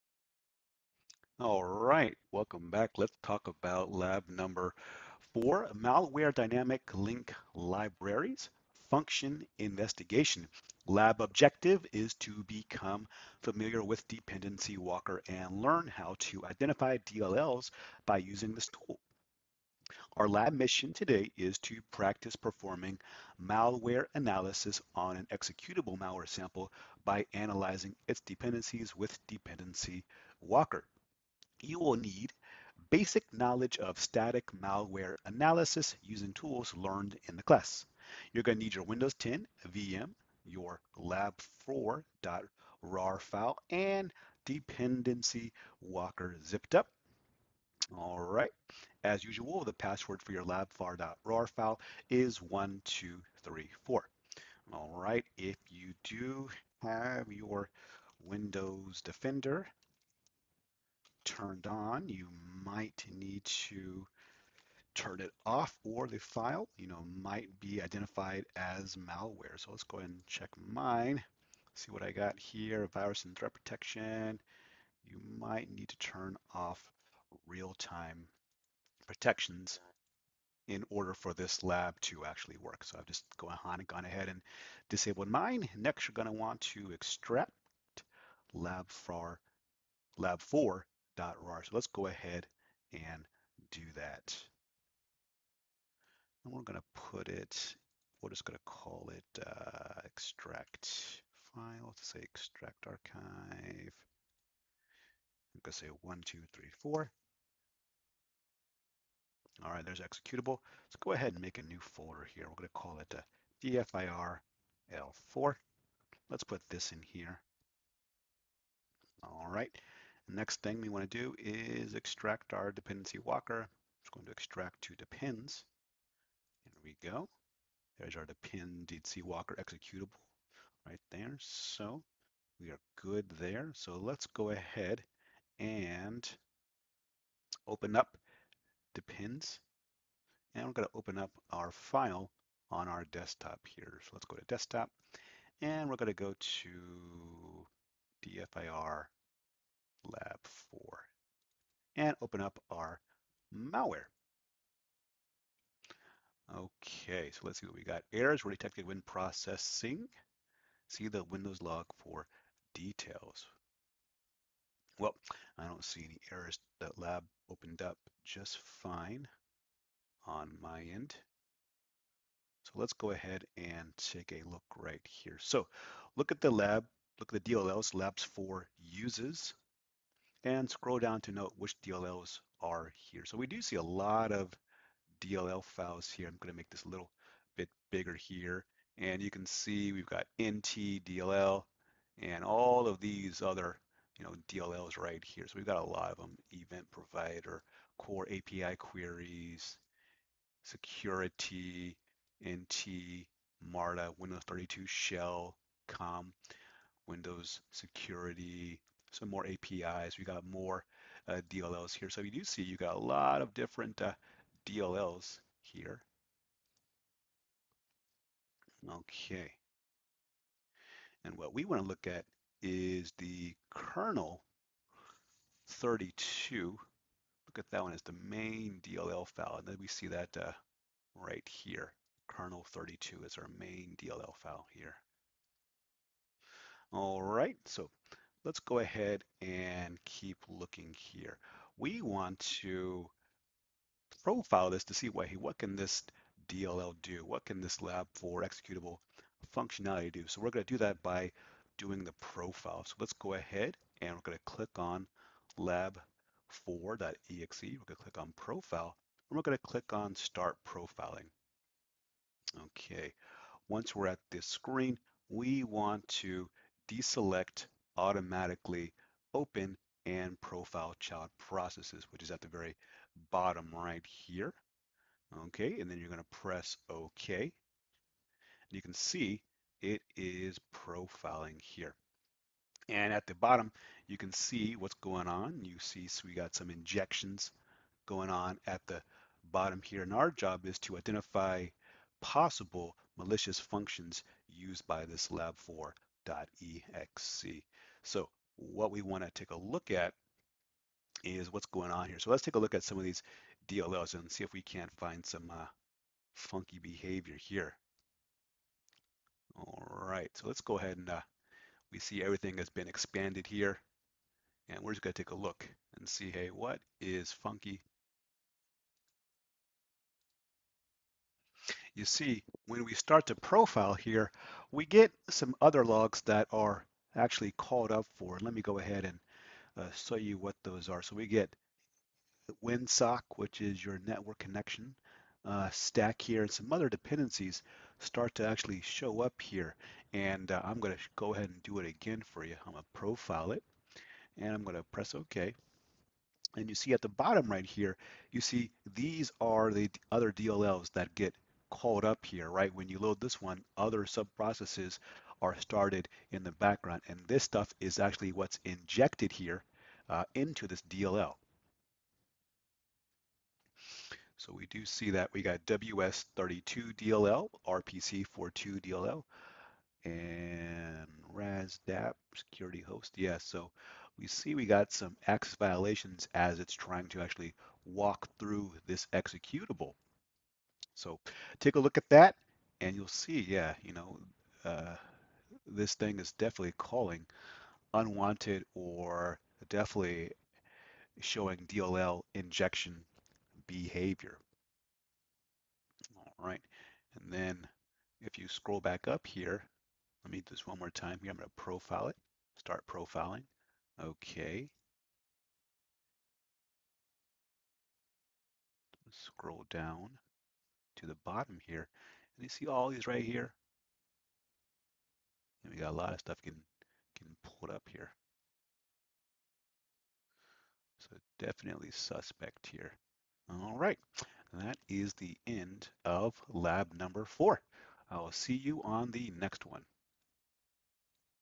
Searching for Malware in DLL's and Portable Executables using Dependency Walker
Recorded using Capto